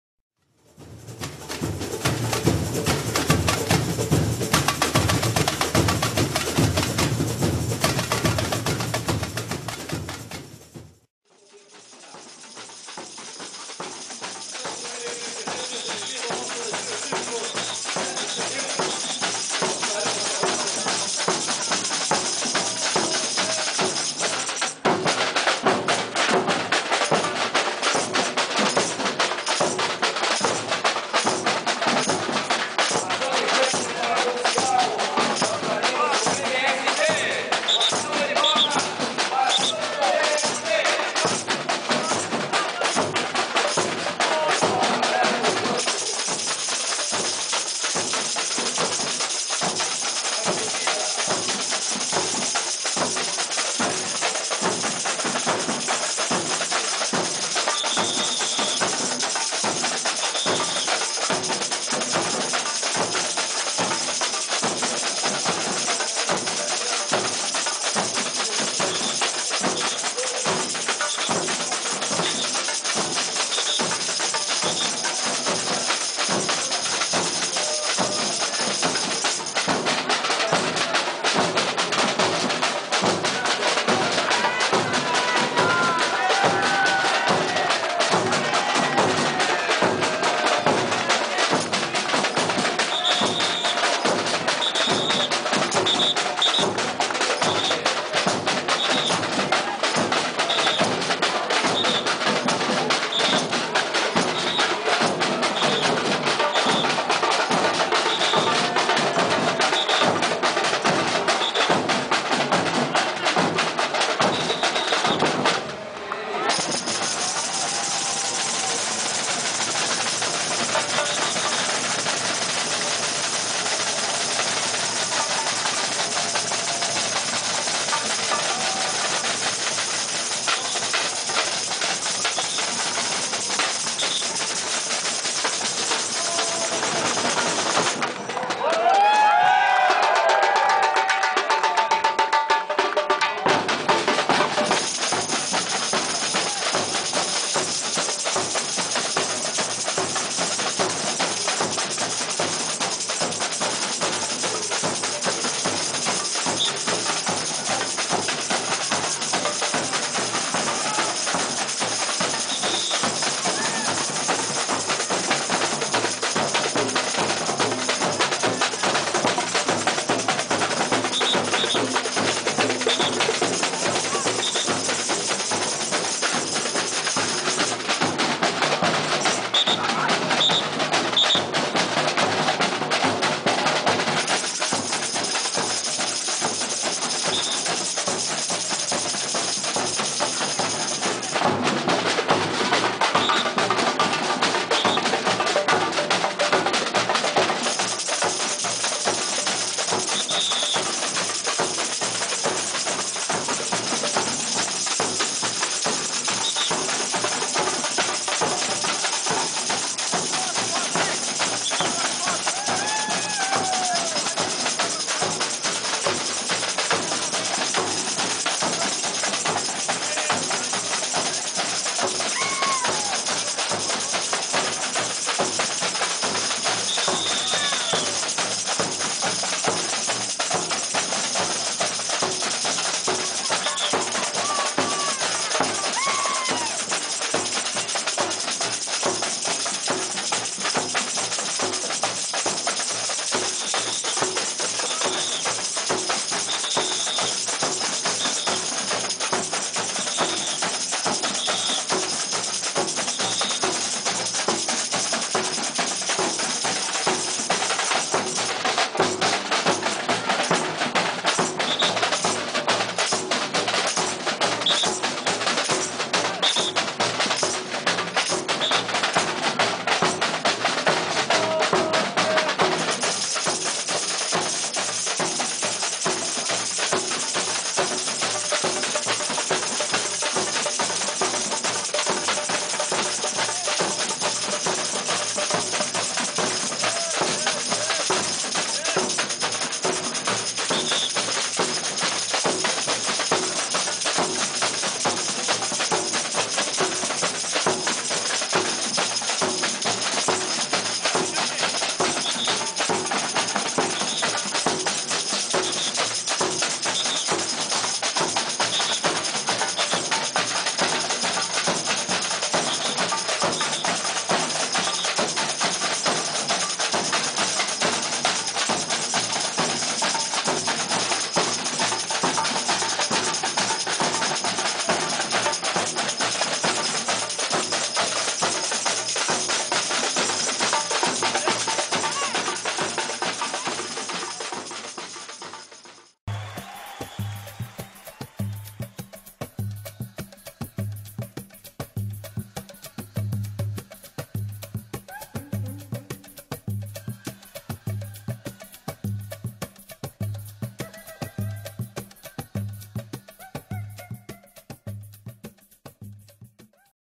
Kadência Karioca .Vídeos Desfile de 7 de Setembro 2011 .avi
Um dos vídeos que registramos no dia 7 de setembro onde participamos do desfile, fomos aplaudidos.
Agora uma de nossas metas é formar mais integrantes e assim fortalecer a nossa bateria.